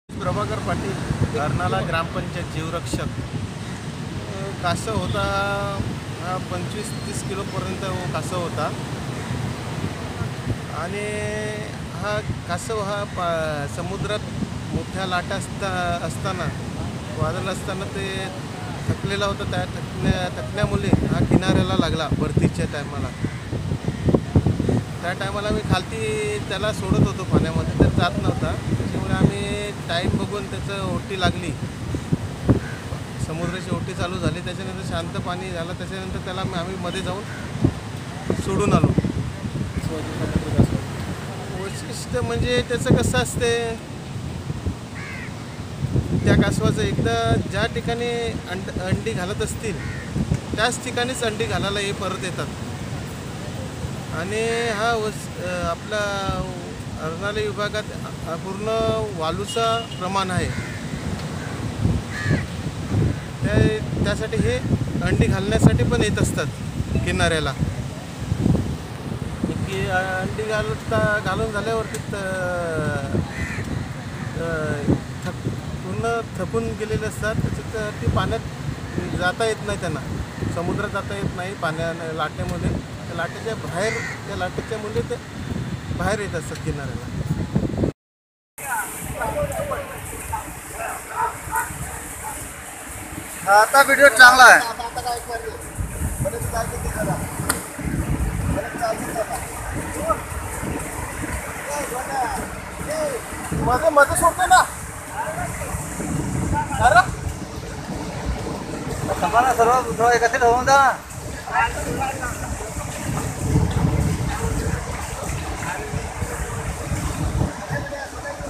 विरारच्या अर्नाळा समुद्रात दुर्मिळ कासव सापडला ,सुमारे 25 ते 30 किलोचा हा
समुद्रातील भरतीच्या पाण्यामुळे अर्नाळा येथील  नवापूर  समुद्रकिनारी  सागरी कासवाचे दर्शन झाले. सुमारे  25 ते 30 किलोचा हा  कासव असल्याचे ग्रामस्थांनी सांगितले.  बुधवारी दुपारच्या सुमारास  समुद्राच्या लाटांमुळे हा कासव वाहत आला, ग्रामस्थानी त्याला तपकडून  पुन्हा समुद्रात सोडण्याचे प्रयत्न करण्यात आले. मात्र भरतीच्या वेळी ते शक्य नसल्याने गुरूवारी  सकाळी आहोटीच्या वेळी कासवाला पुन्हा  समुद्रात सुखरूप सोडण्यात आले.हे कासव दुर्मिळ जमातीचे असल्याचे तटरक्षक मनोज पाटील यांनी सांगितले